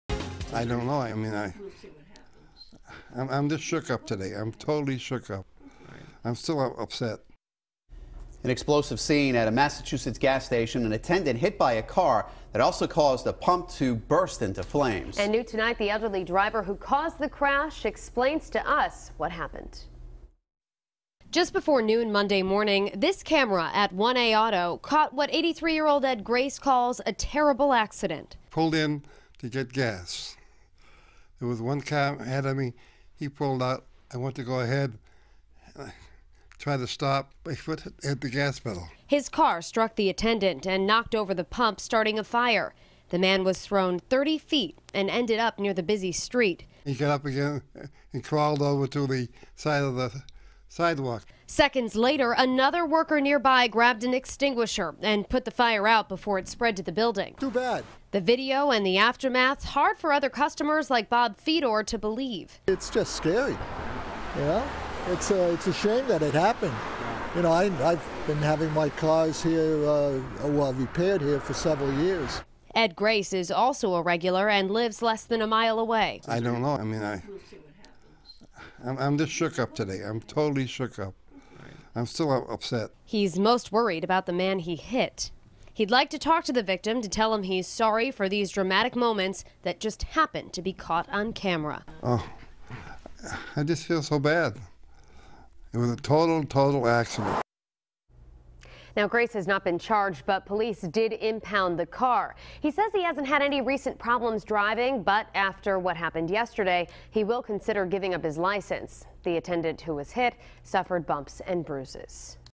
Attendant hit, pump bursts into flames
A Providence man is recovering at home after he was hit by a car at his own job. The entire accident was caught on camera.